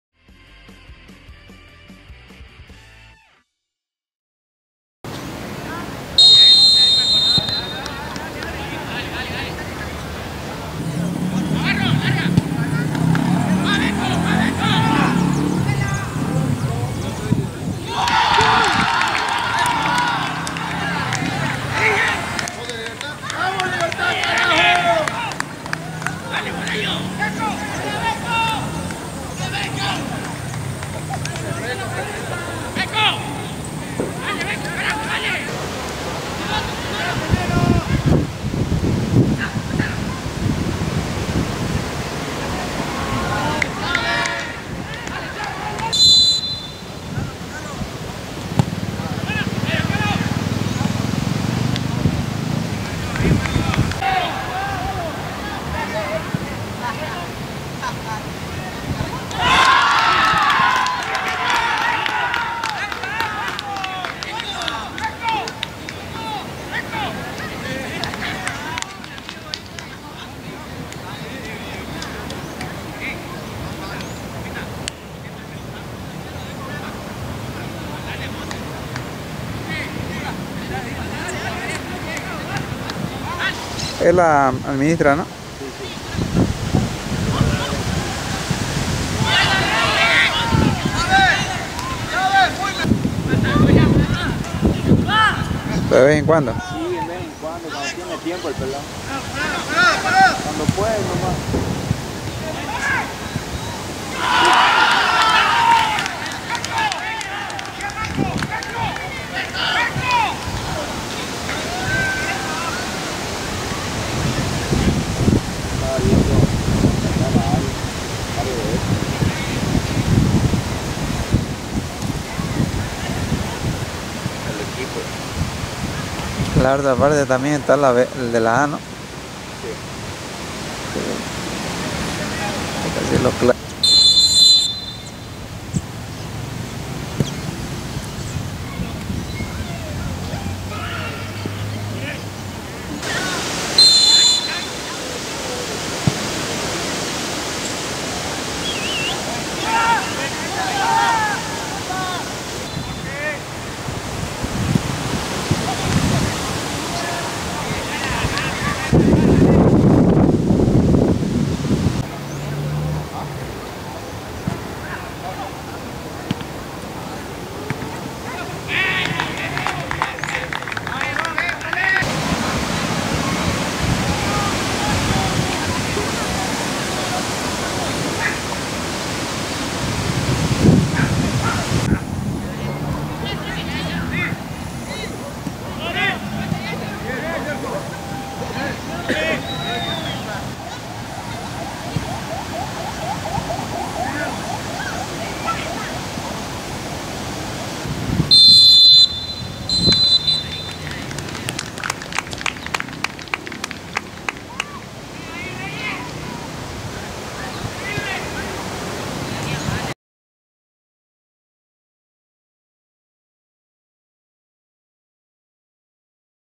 LIBERTAD (3- 0) TAMAYA JIMENEZ
TORNEO DE LA DIVISIÓN "1RA." ASCENSO 2022 FECHA # 5, CANCHA LIBERTAD
LIBERTAD (3- 0) TAMAYA JIMÉNEZ